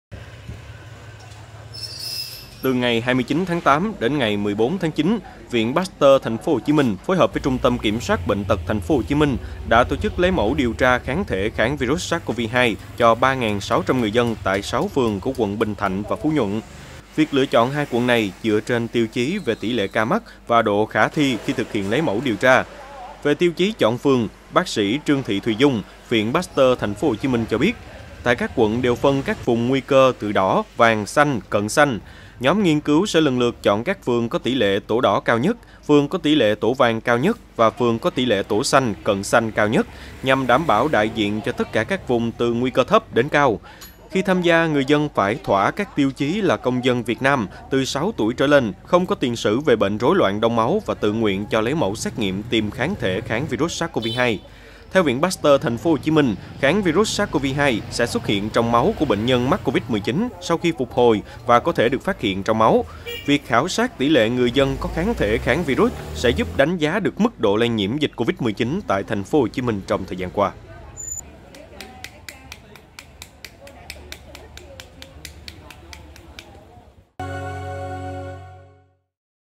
Lấy mẫu 3.600 người dân tại TP.HCM để điều tra kháng thể kháng virus SARS-CoV-2
Từ 29-8 đến 14-9 Viện Pasteur TP.HCM phối hợp với Trung tâm kiểm soát bệnh tật TP.HCM đã tổ chức lấy mẫu điều tra kháng thể kháng virus SARS-CoV-2 cho 3.600 người dân tại 6 phường của quận Bình Thạnh và Phú Nhuận.